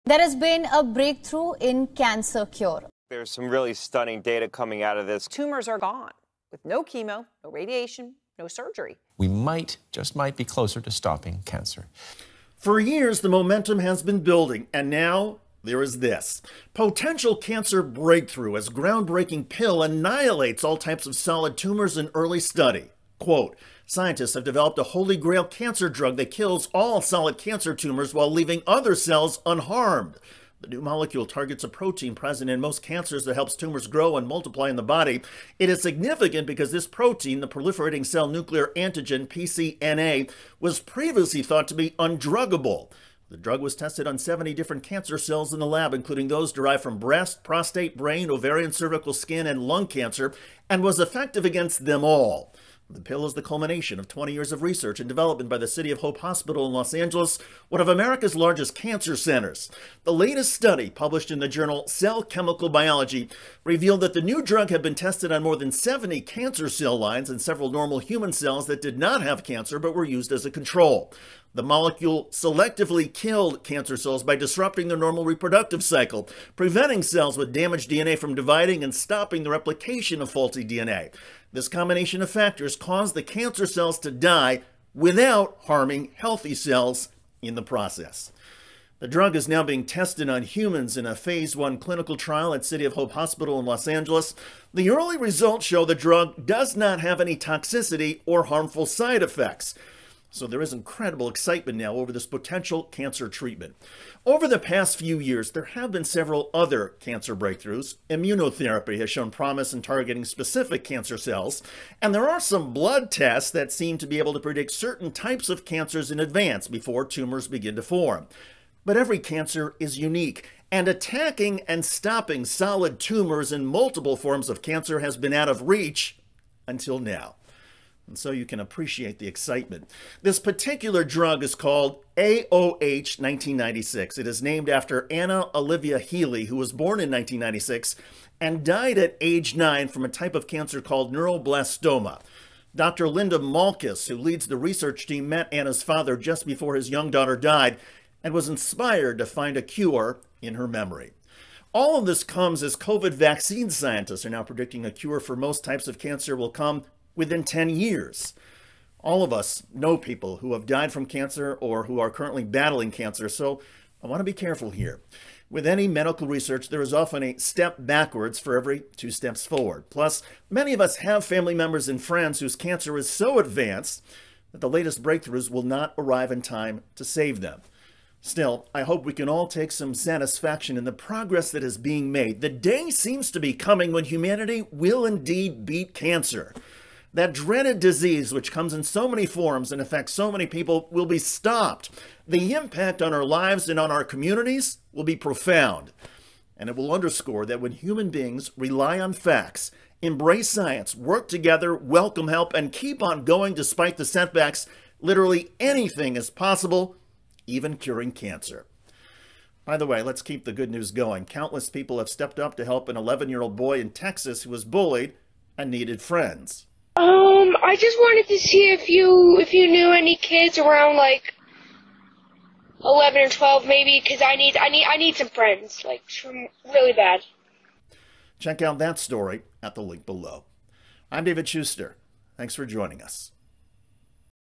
Cancer Cure Breakthrough Revealed By Scientists
Watch: Bullied Boy Needed Friends, Countless Strangers Step Up

Scientists have developed a ‘holy grail’ molecule that kills all solid cancer tumors, leaving healthy cells unaffected. David Shuster breaks it down on Rebel HQ.